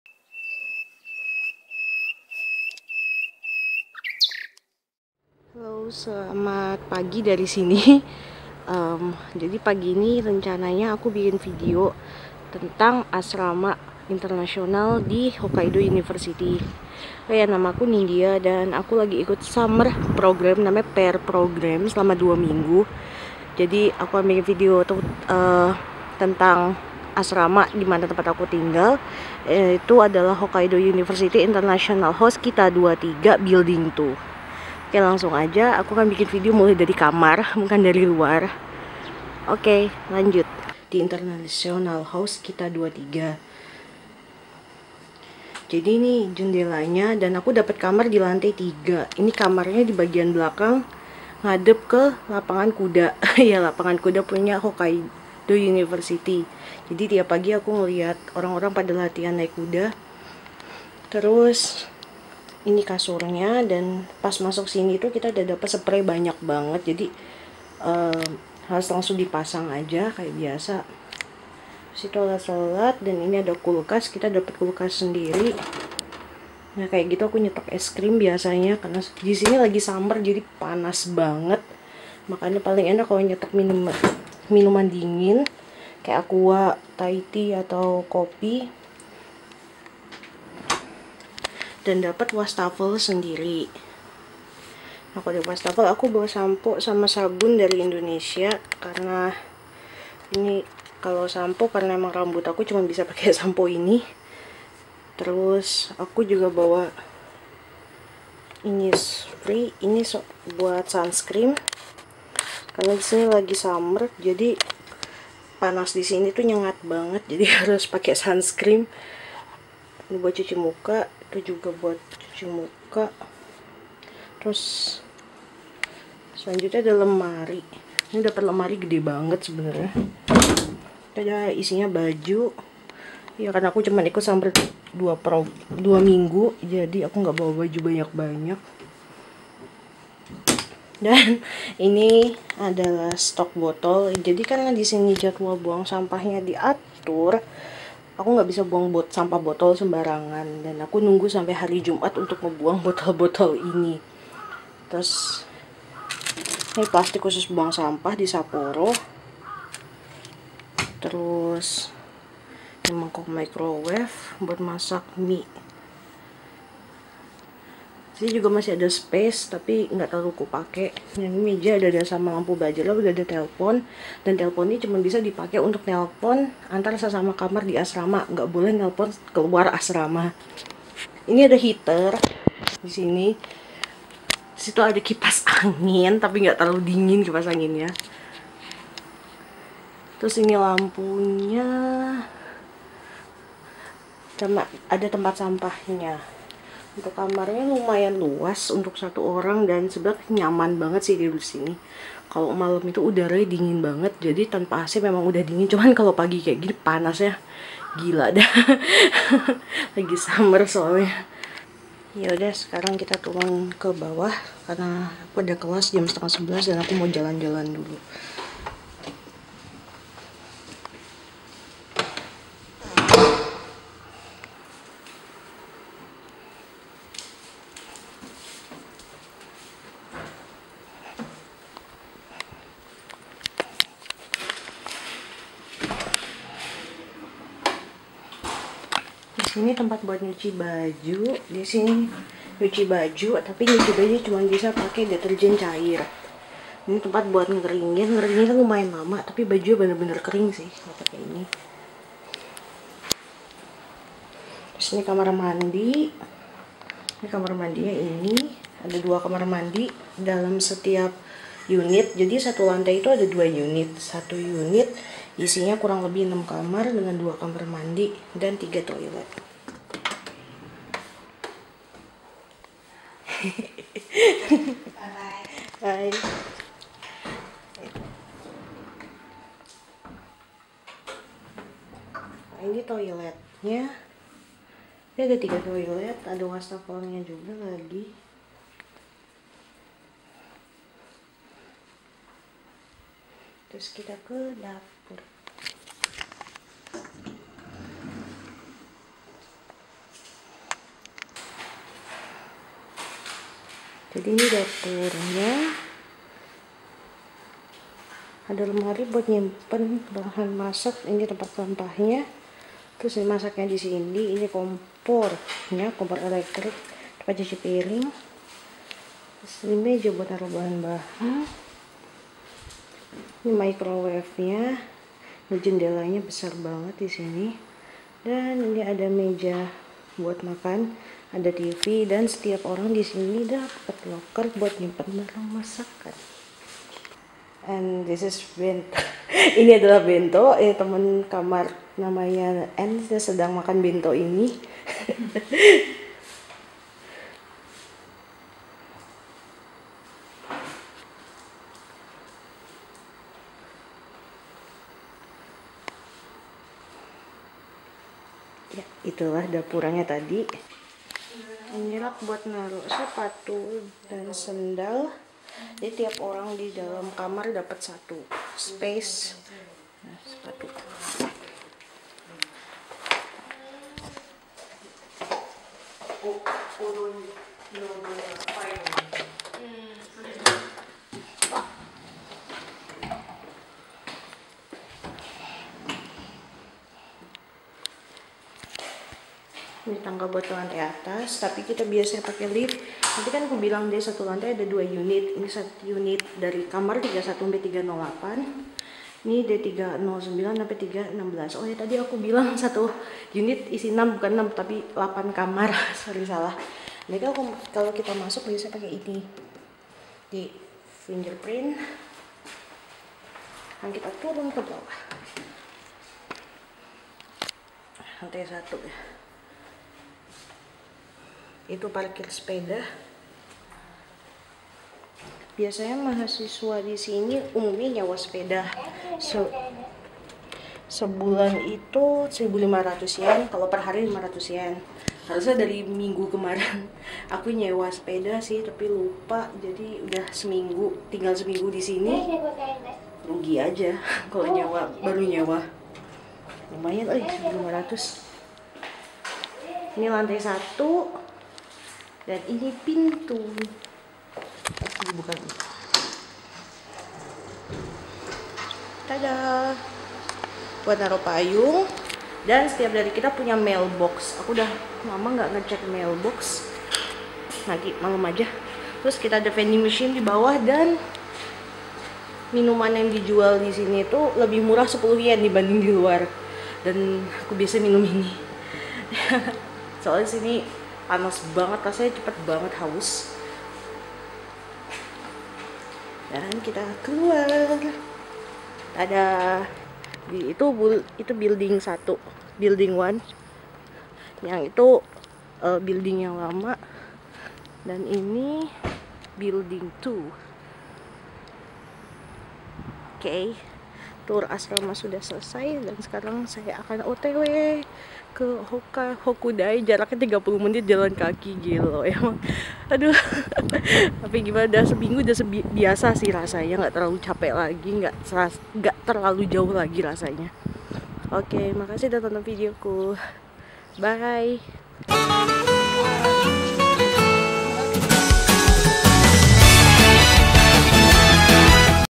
Tur Asrama : Hokkaido University International House Kita 23
Hallo~
Kali ini aku bikin video tur asrama internasional Hokkaido University. Jadi selama summer school aku tinggal di asrama ini bersama dengan mahasiswi internasional lainnya.